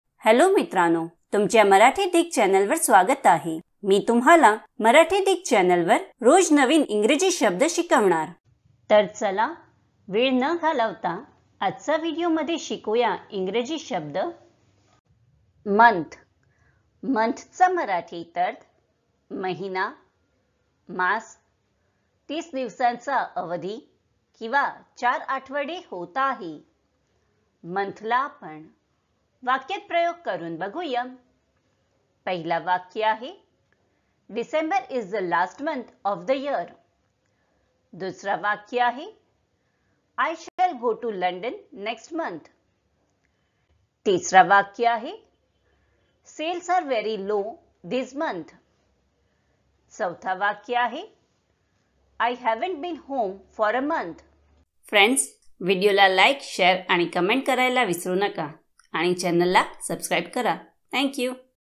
Month Meaning in Marathi | Month म्हणजे काय | Month in Marathi Dictionary
या व्हिडिओमध्ये तुम्हाला मराठीत Month चा अर्थ समजेल आणि Month चा उच्चारही शिकाल. याचा अर्थ तुम्ही Month चा अर्थ जाणून घ्याल तसेच Month कसे उच्चारता येईल, Month म्हणण्याचा योग्य मार्ग कोणता आहे.

तर Month शब्द फक्त एका मिनिटात शिका.

उदाहरणांसह Month समजून घेऊ.